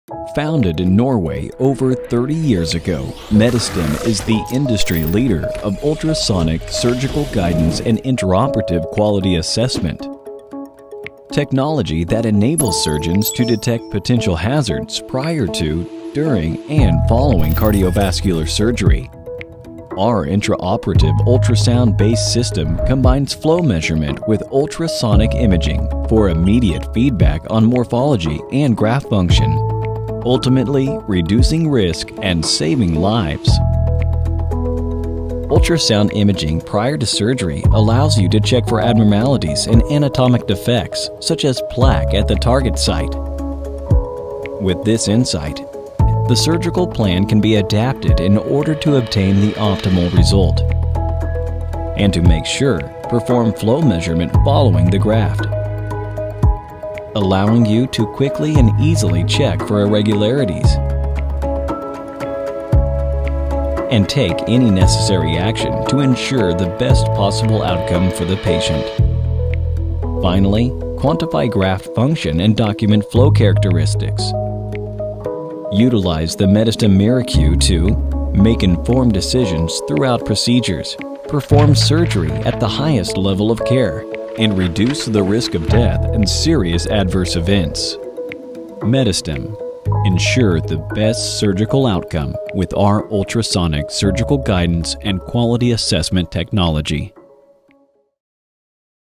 Medistim Introduction Video
Learn how intraoperative transit time flow measurement (TTFM) combined with ultrasonic imaging can help ensure the best surgical outcome during Cardiac, Vascular and Transplant surgery